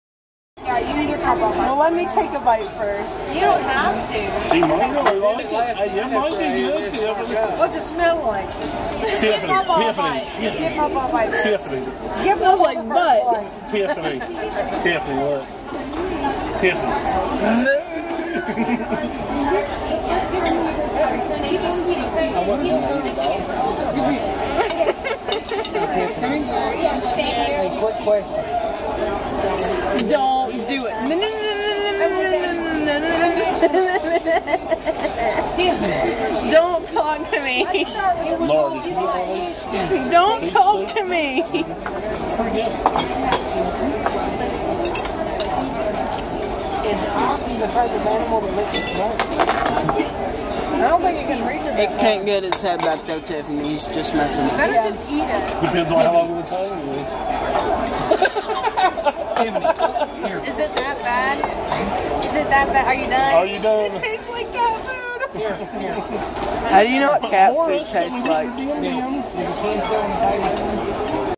I ate BRAISED OX TONGUE?
While on the cruise, one of our appetizer choices at dinner was braised ox tongue. My uncle offered me $10 to eat the whole thing. Watch to see how it turned out.